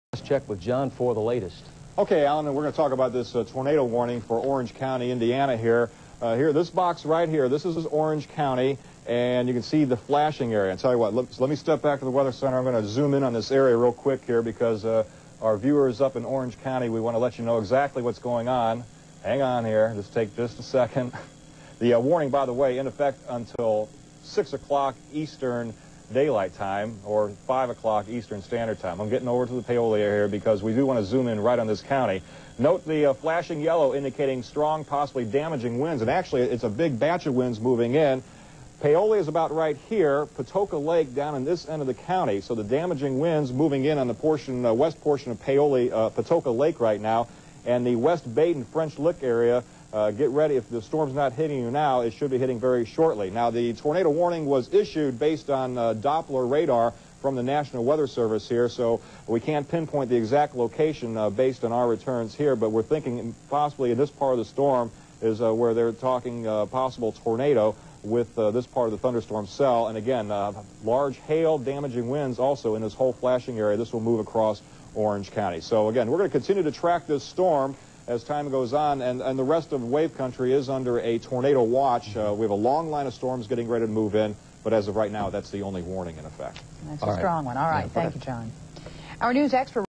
WAVE-TV 1995: Orange County Tornado Warning
A clip from WAVE-TV from 1995 featuring meteorologist John Belski updating viewers on a tornado warning for Orange County, IN and zooming in on the storm cell using the radar while doing so